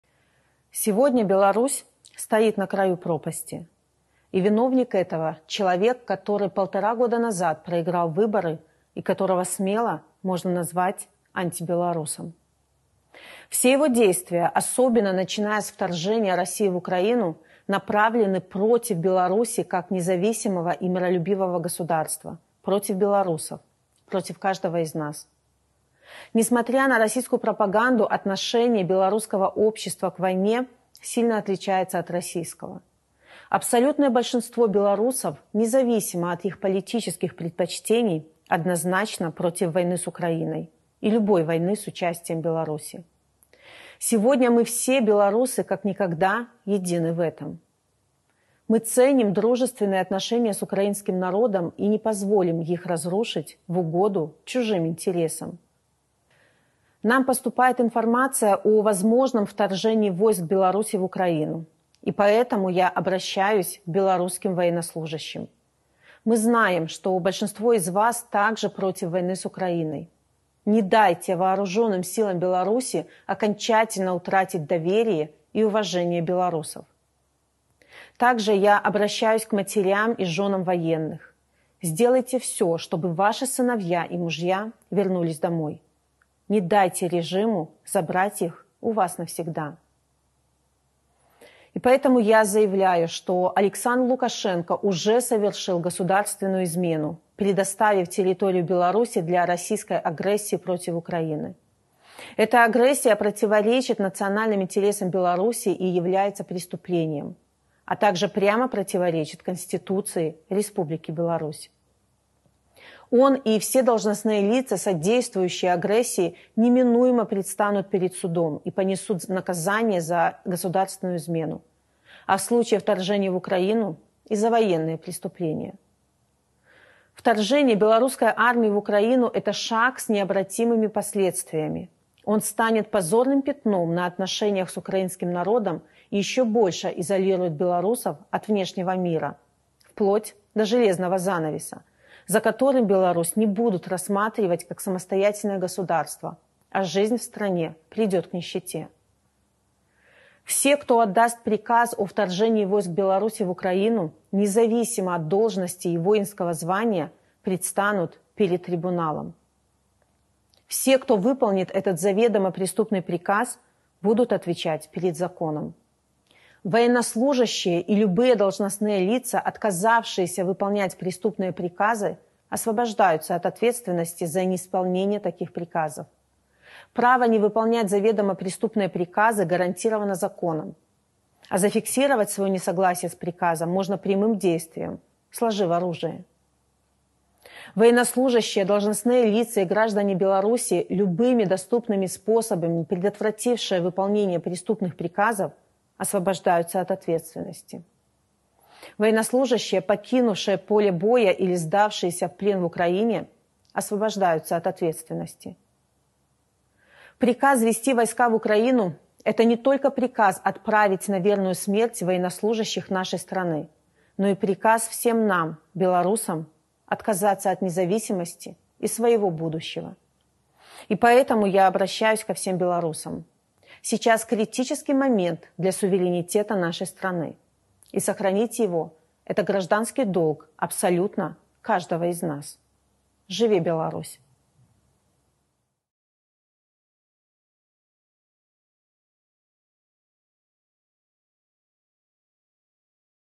Світлана Тихановська опублікувала звернення до білоруських військовослужбовців
Лідерка білоруської опозиції Світлана Тихановська опублікувала звернення до білоруських військовослужбовців у зв’язку з появою інформації про можливе вторгнення військ білорусі в Україну — закликає білоруських військових здаватися в полон Україні.